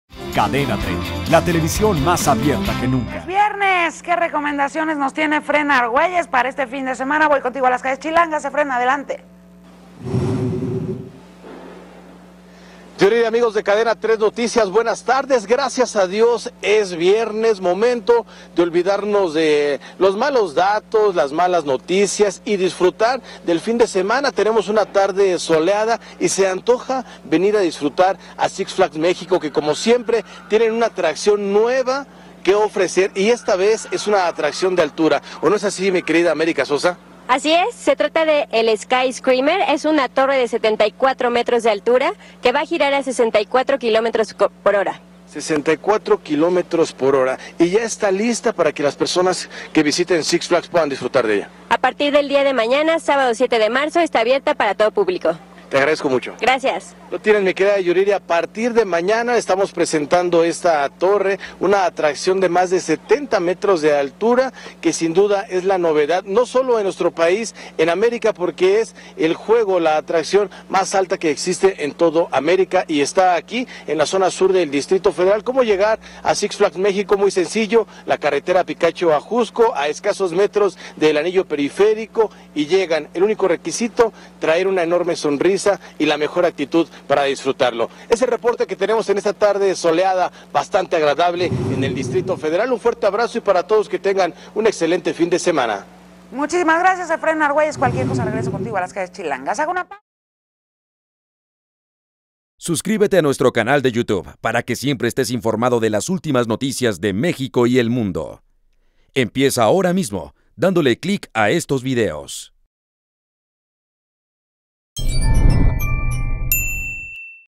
Reta al vértigo con Sky Screamer, la nueva atracción de Six Flags
Six Flags México inauguró su nueva atracción llamada Sky Screamer, una torre giratoria de 74 metros de altura, la más alta de América Latina, que eleva a los usuarios a una velocidad de 64 kilómetros por hora durante dos minutos. 06 marzo de 2015